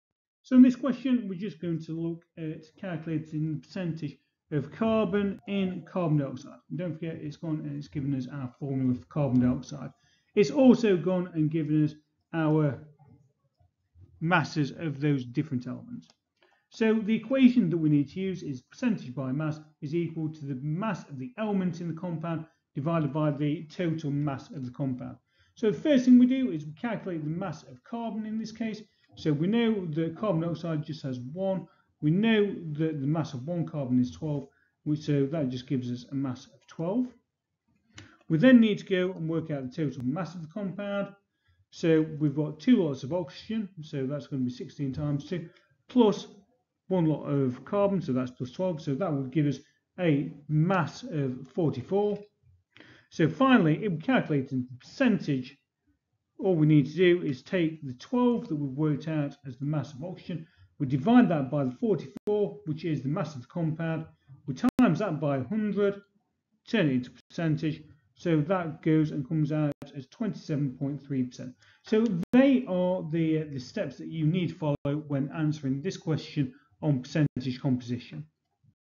GCSE Chemistry: Calculating percentage composition video 1
Mastering Percentage Composition Calculations in GCSE Chemistry! 📊 Dive into our comprehensive tutorial on calculating percentage composition in chemical compounds. 🤔 Explore step-by-step methods, formulas, and practical examples to understand how to determine the percentage by mass of elements in compounds. Let's demystify chemical composition calculations together!